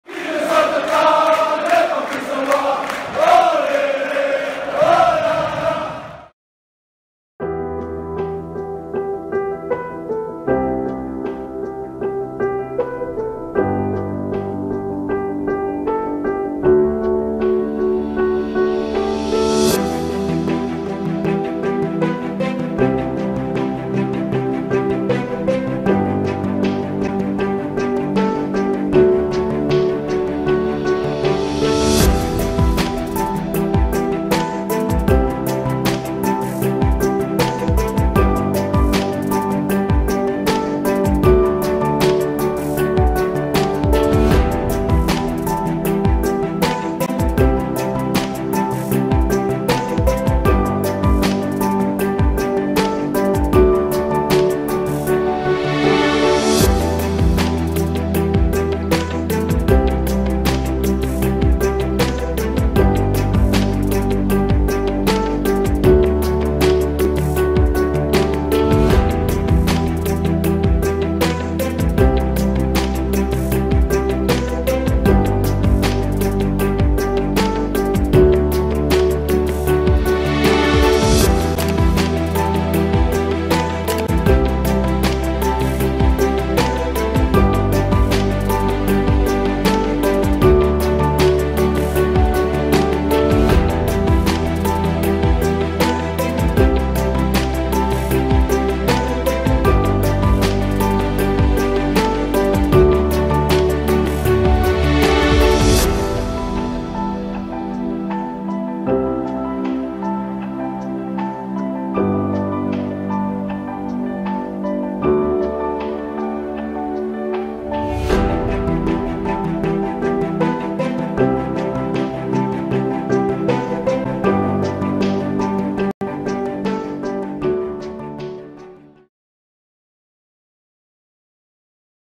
#14 // RSC Anderlecht // Lotto Park
Club // RSC Anderlecht
Stadium // Lotto Park (Constant Vanden Stockstadion)
Capacity // 21.500 Seats

Location // Anderlecht, Belgium